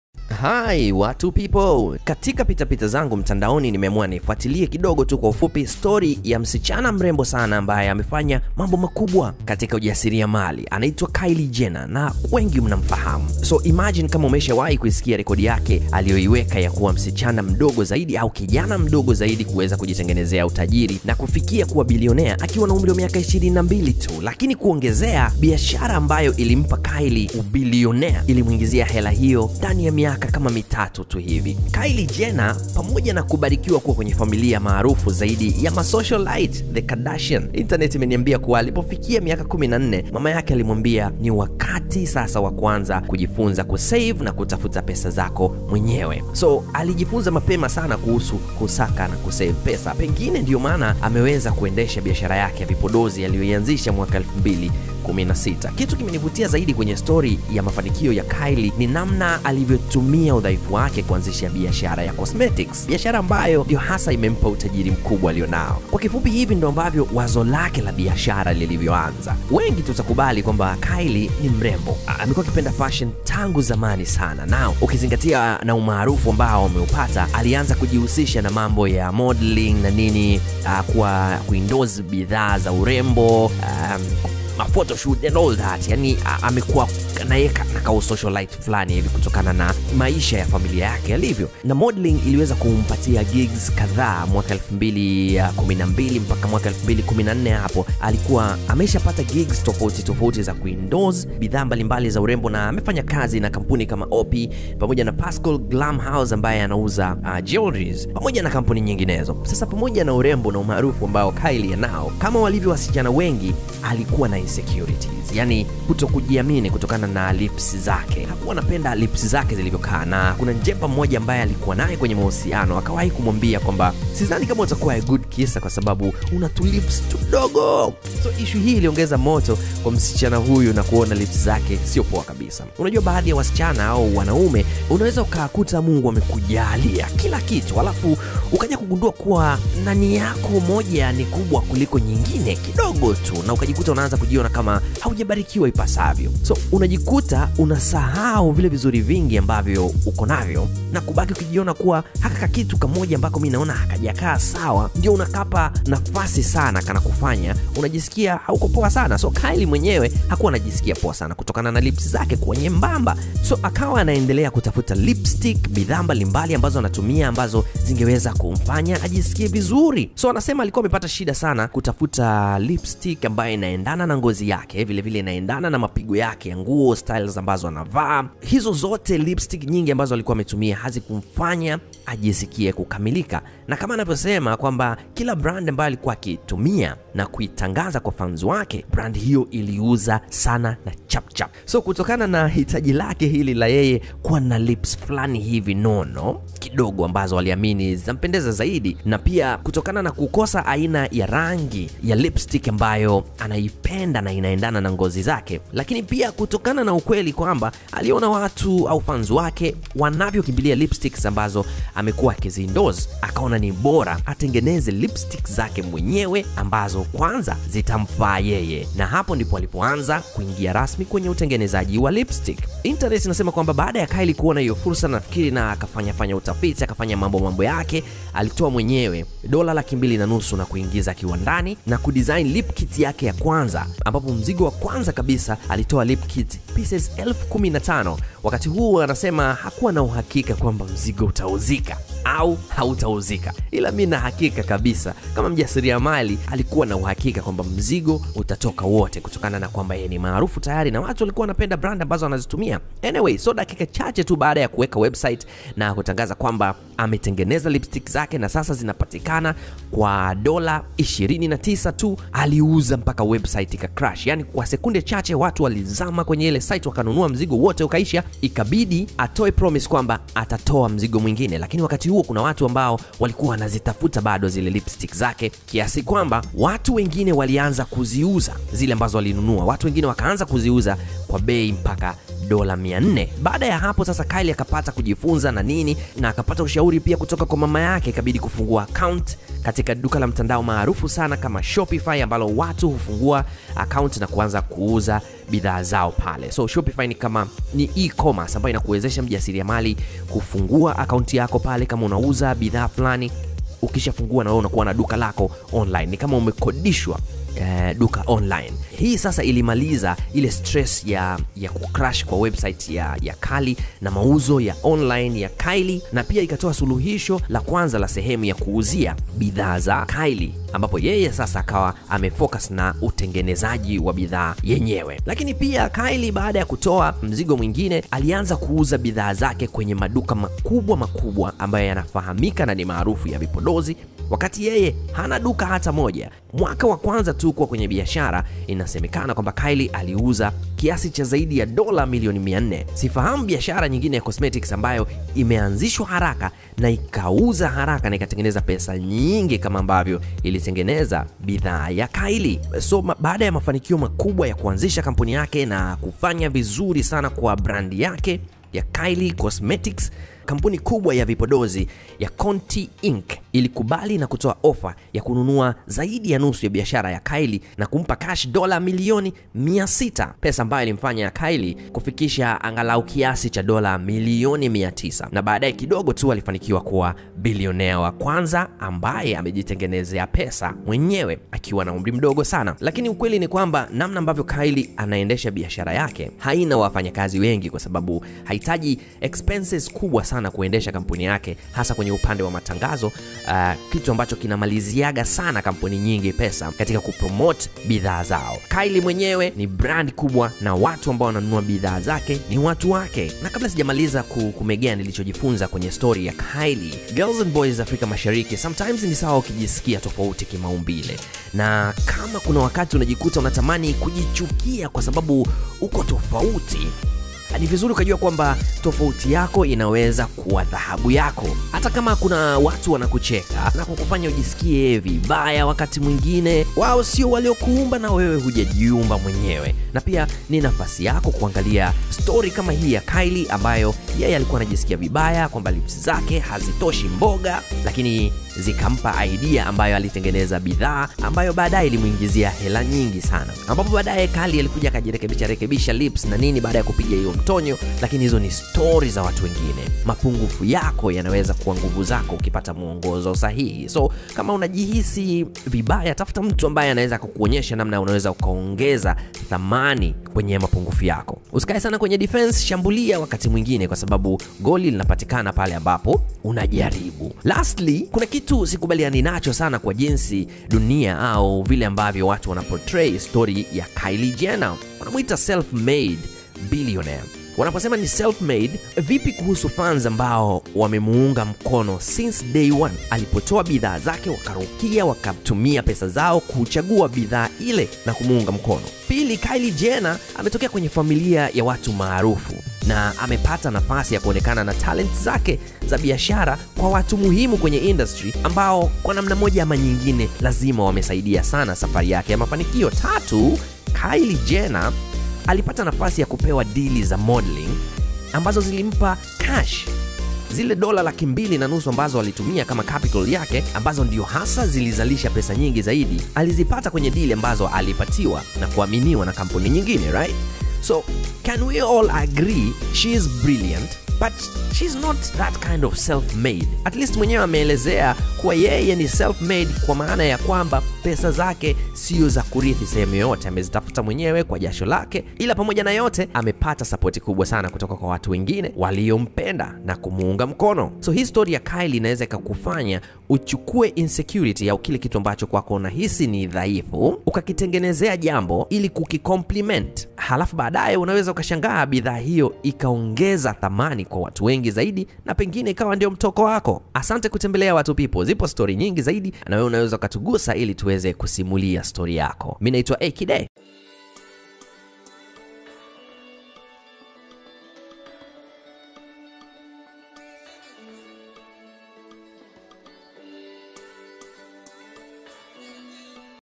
HOW TO OVERCOME INSECURITIES AND BUILD BUSINESESS LIKE KYLIE JENNER
A short story about how a business smart lady Kylie Jenner started her cosmetic business and she managed to become the first youngest billionaire in the world. Stori ya Kylie inatufundisha how bullying and insecurities can be taken converted in to positive thoughts or ideas. How to become Kylie Jenner